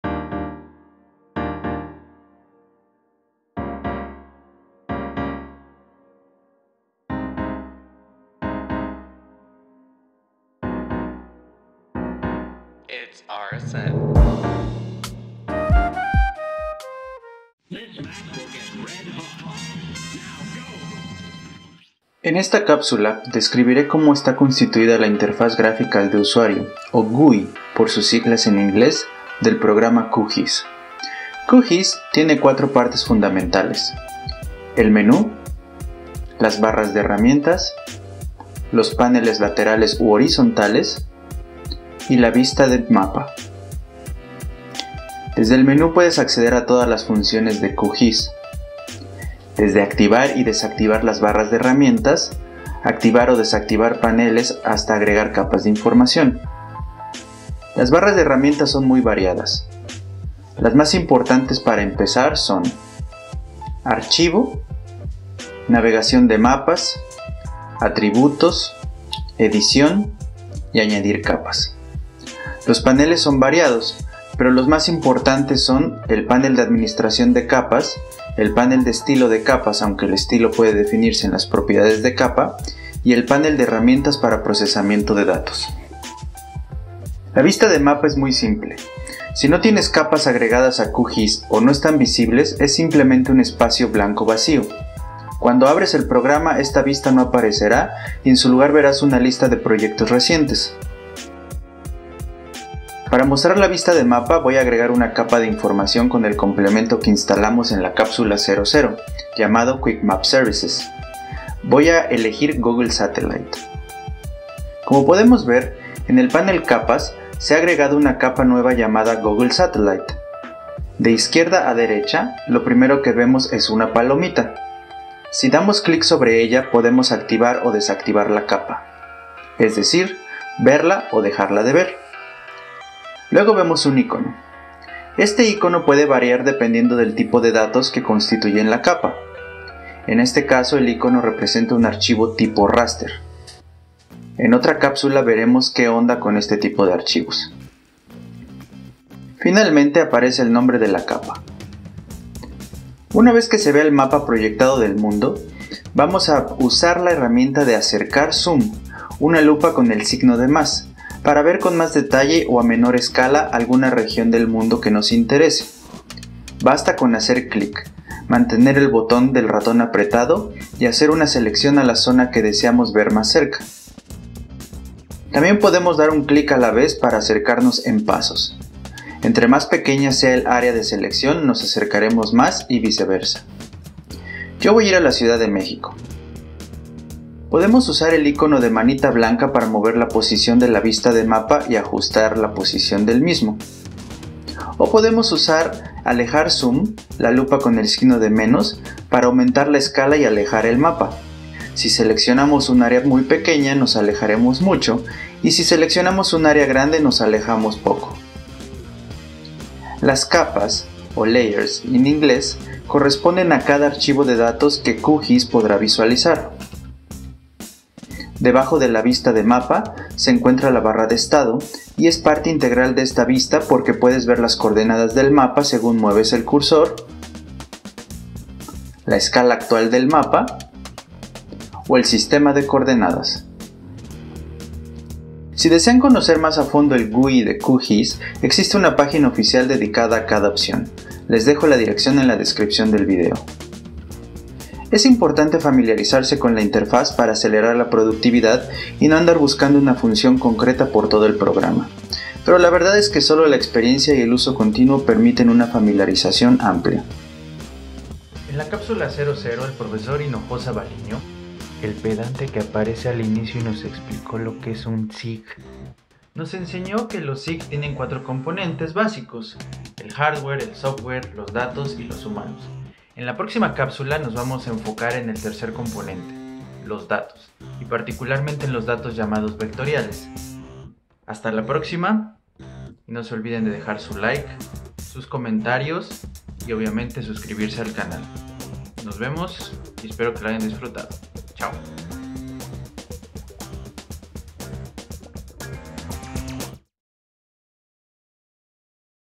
#01 SIG en 5 - GUI de QGIS
GUI de QGIS
Interfaz Gráfica de Usuario / Graphical User Interface
Menús, Paneles, Herramientas.

Lower Third Titles (Títulos de inicio) diseñada y distribuida por Saad Shah - Gargoyles At Work, 2020. Youtube